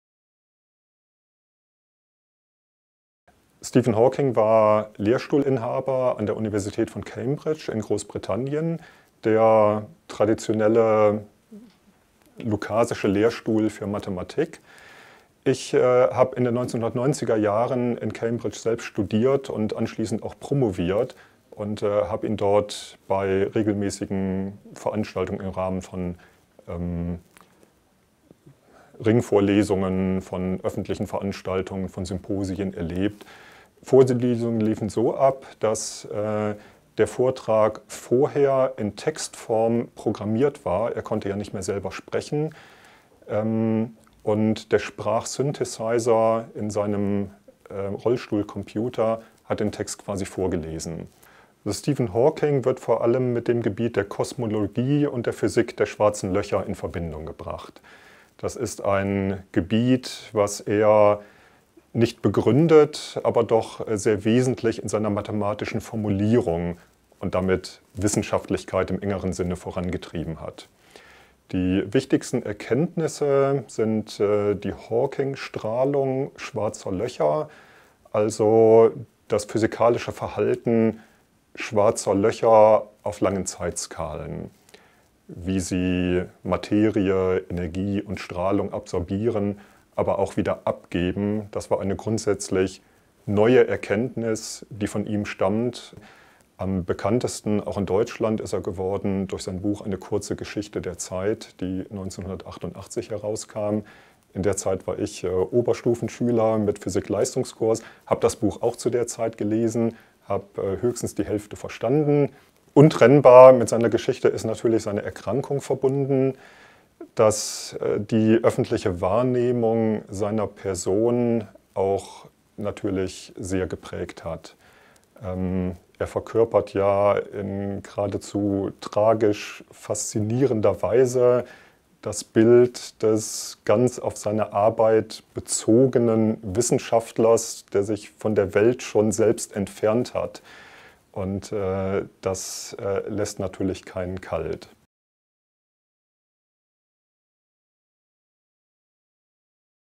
UPB erklärt – Prof. Dr. Arno Schindlmayr zu Stephen Hawking
Im Frühjahr 2018 starb der weltbekannte Physiker Stephen Hawking. Prof. Dr. Arno Schindlmayr vom Department Physik der Uni Paderborn erlebte den Engländer während seines Studiums an der University of Cambridge aus nächster Nähe in Vorlesungen. Im Interview blickt er auf diese Zeit zurück, erklärt, welche großen wissenschaftlichen Leistungen von Hawking bleiben und warum er über die Fachwelt hinaus Menschen faszinierte.

Produktion: Stabsstelle Presse und Kommunikation